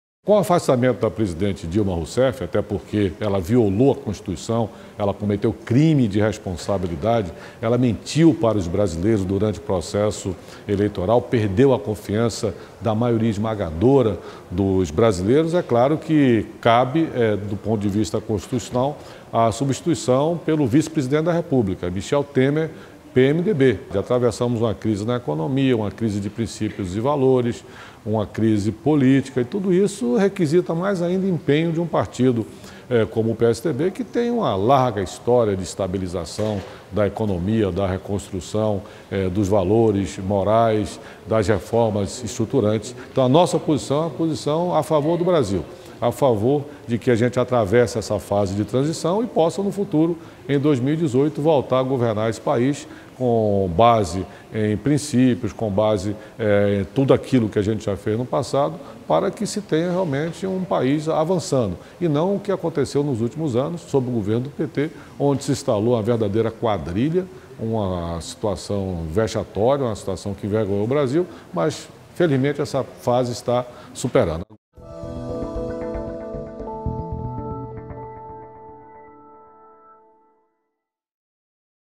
Imbassahy: reconstruir o país depois da quadrilha da era PT
O Brasil herdou, de Dilma e do PT, uma crise econômica e de valores cuja superação exige esforço e equilíbrio, avalia o deputado federal Antonio Imbassahy (PSDB-BA. “Felizmente, está superada a fase dos últimos anos de governo do PT, que envergonharam o país”, diz.